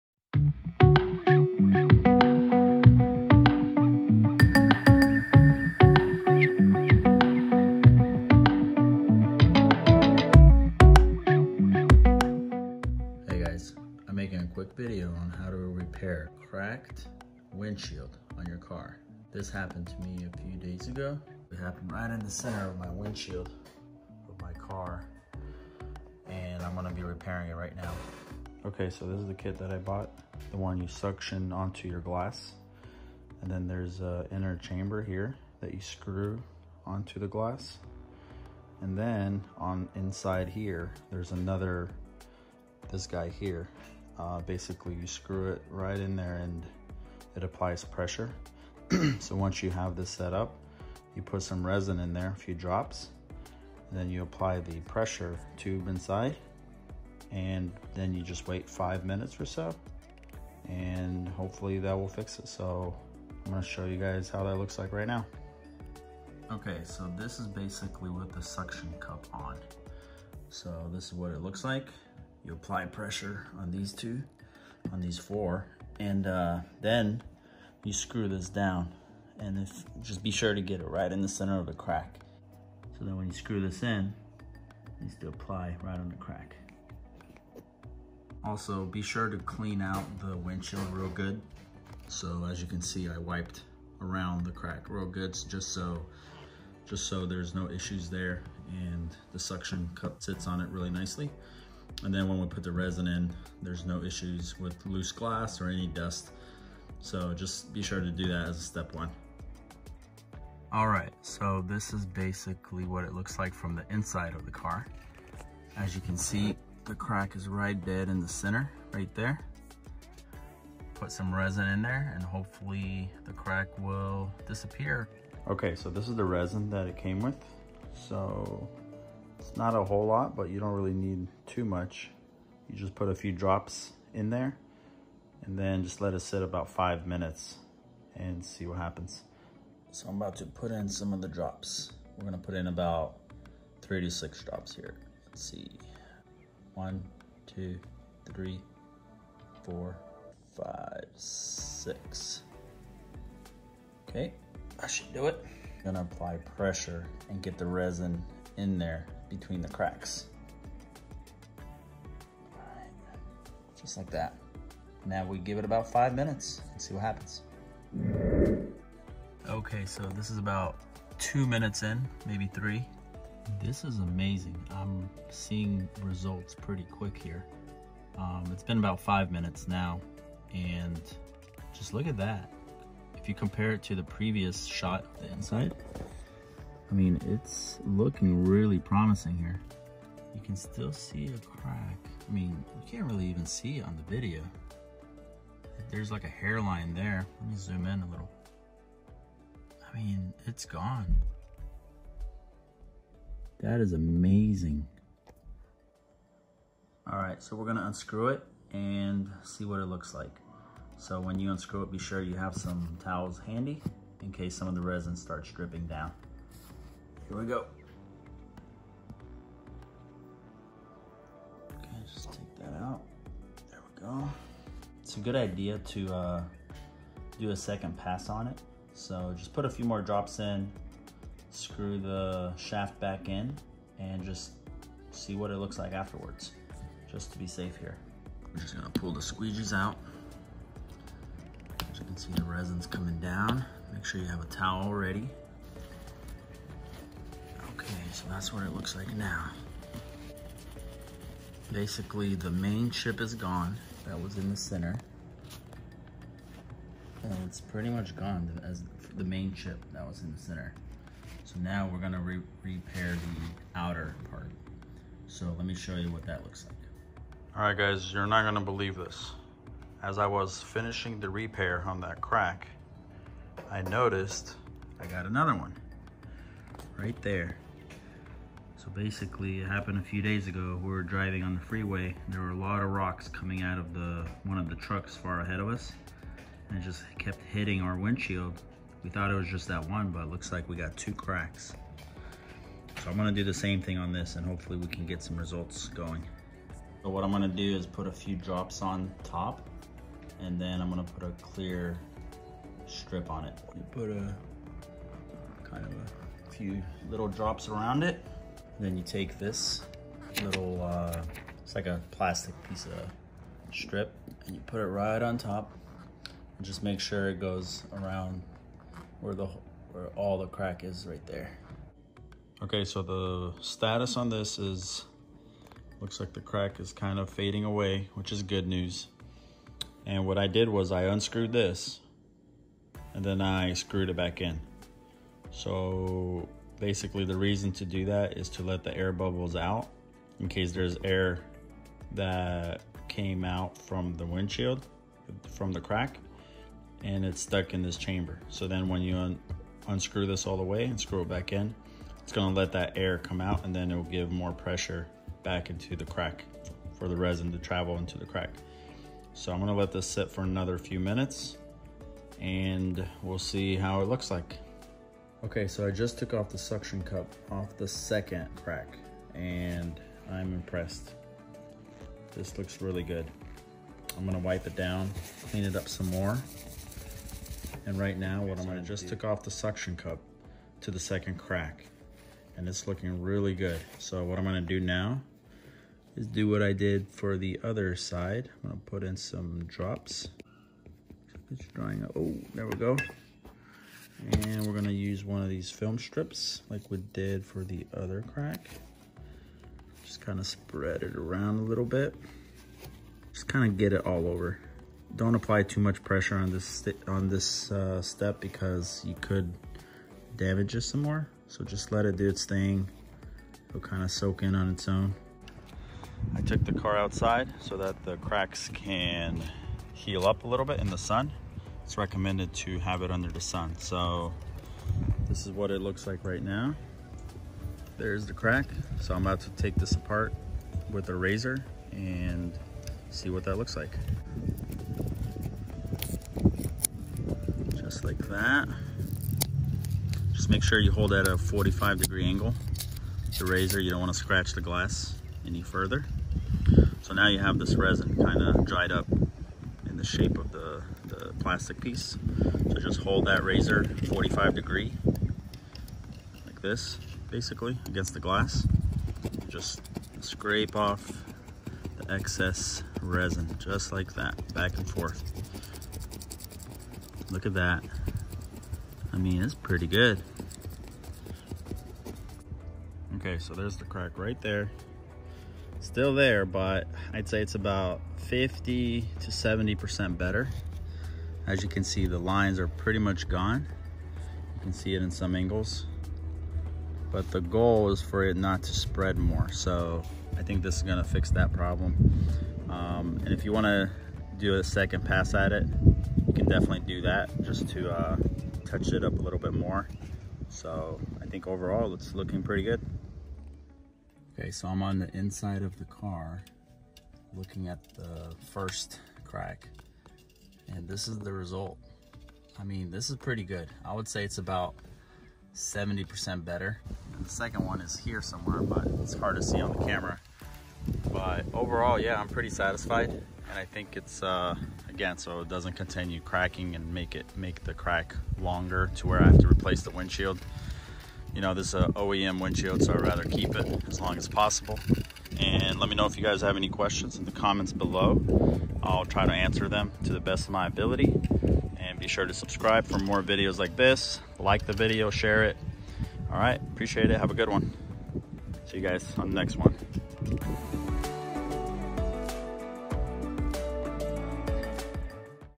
Repair cracked windshield quick and cheap!
Hey there, in this video I will demonstrate the way to fix a crack or chip in your windshield. You'd like to get it done RIGHT when it happens otherwise the crack will spread and you'll have to change out your windshield.

Let me know if you have any questions whatsoever!